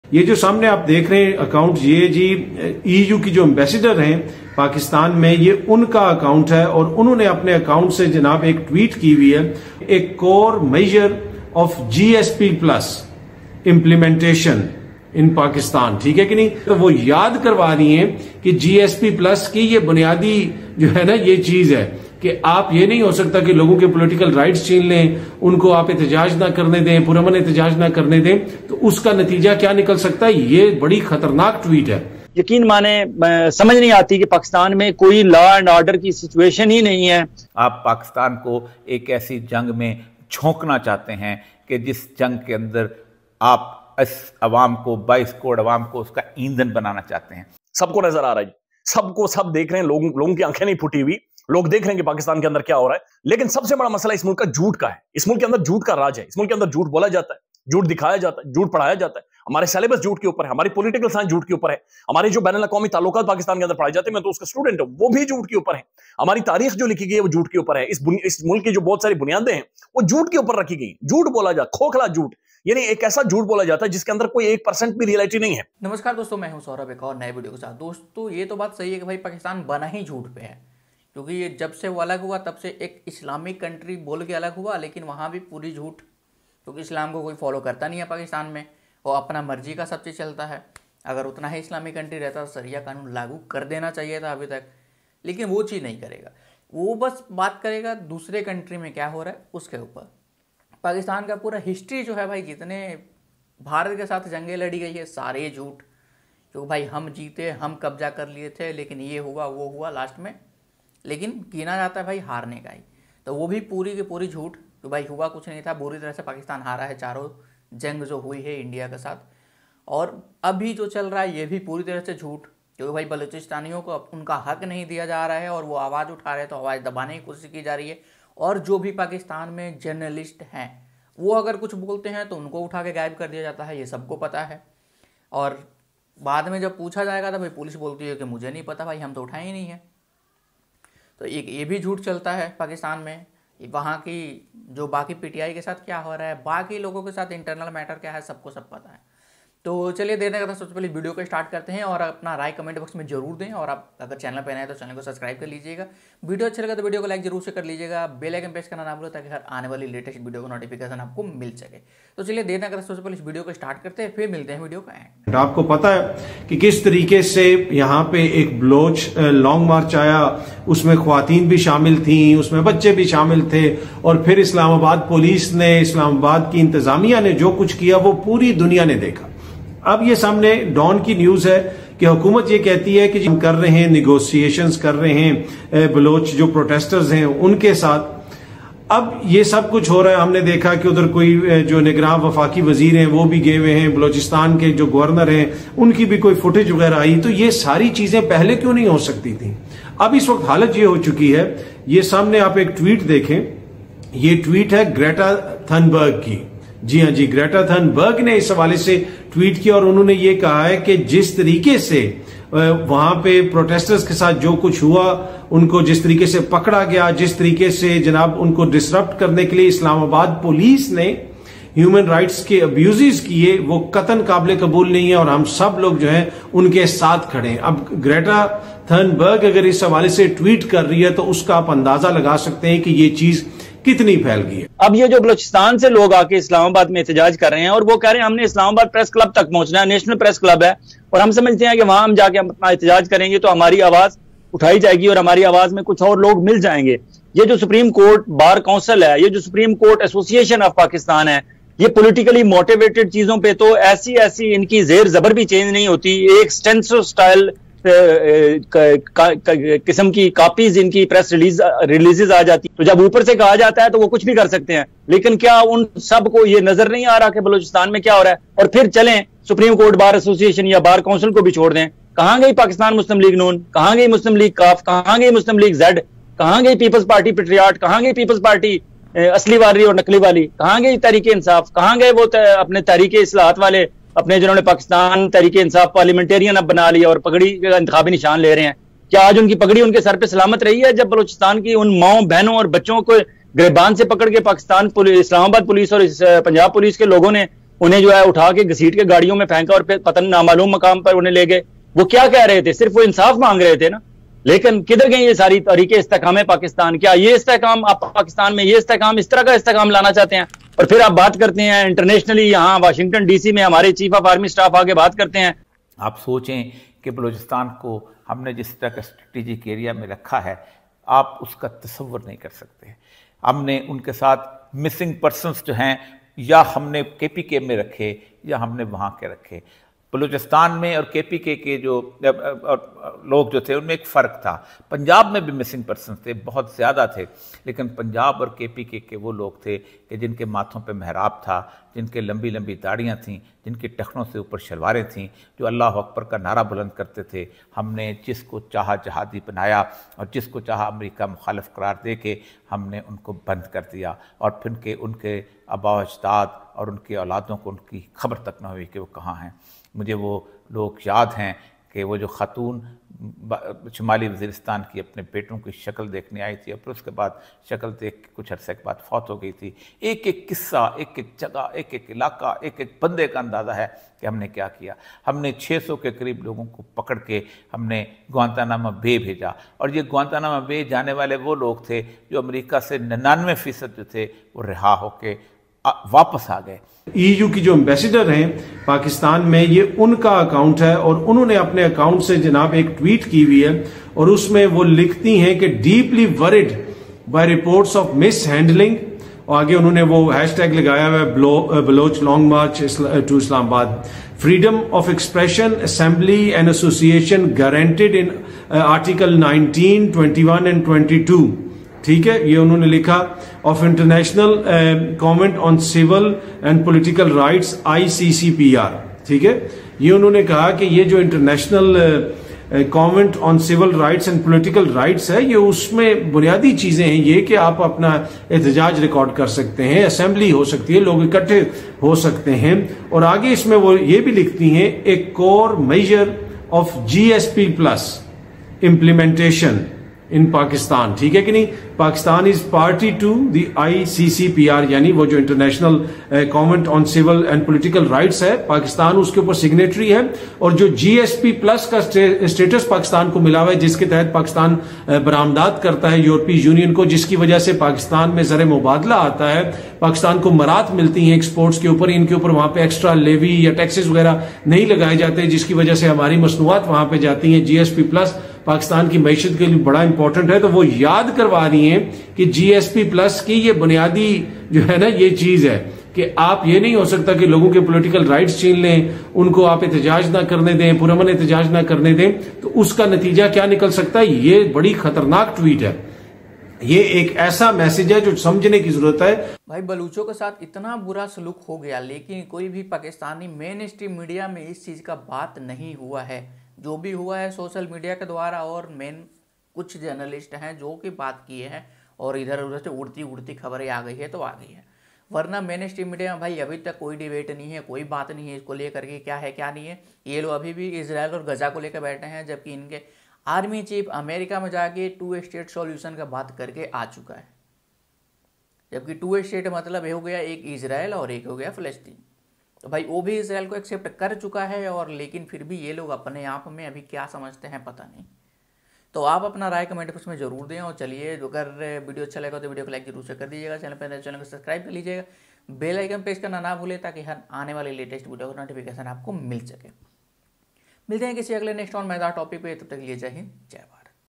EU के एम्बेसडर ने दिखाई Pakistan को उसकी औक़ात | GSP Plus छीनने की दी धमकी पूरा पाक मीडिया रोया 😭
1971 yaad kar fut-fut kar roya pak media diya pak fauj ko baddua 😂